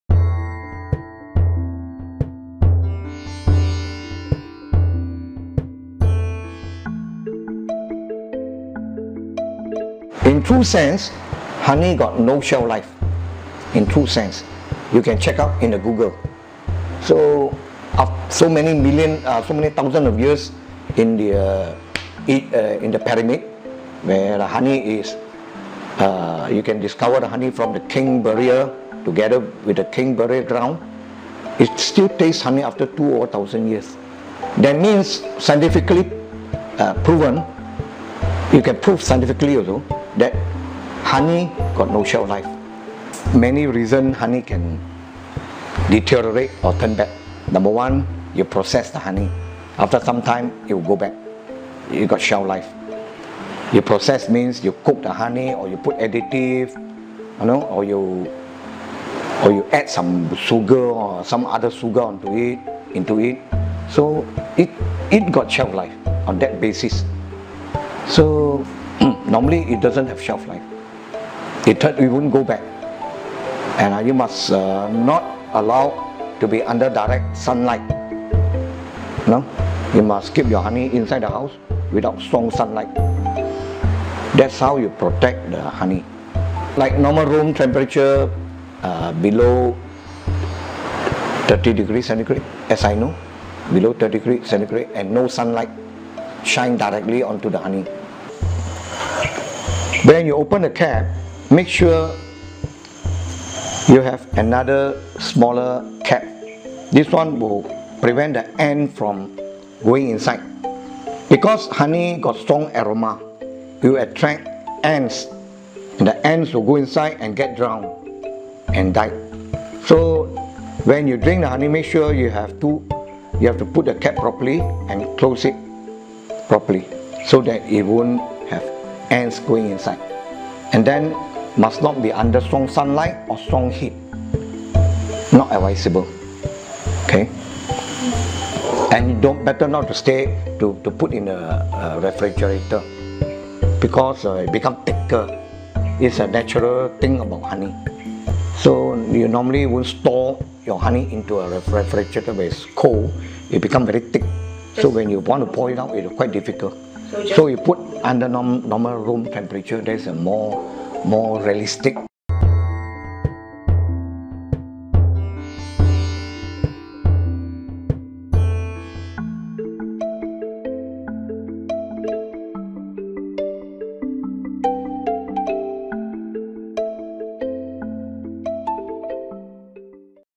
What is the shelf life of Gu Xiang Yuan wild honey? And How to storage?
古乡苑峰岭蜜的保质期是多久？要如何储存？